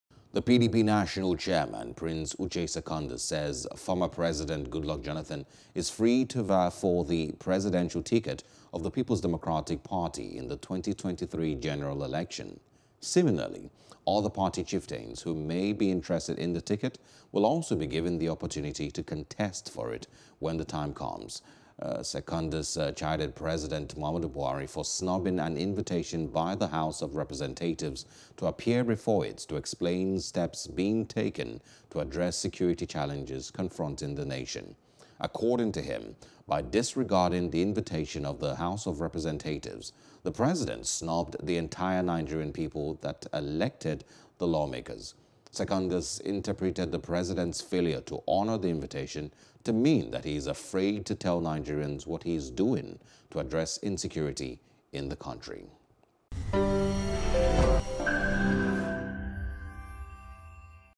Goodluck Jonathan Is Free To Vie For PDP Ticket – PDP National Chairman (News | Nigeria)
PDP National Chairman, Prince Uche Secondus says Former President Goodluck Jonathan is free to vie for the presidential ticket of the People’s Democratic Party in the 2023 general election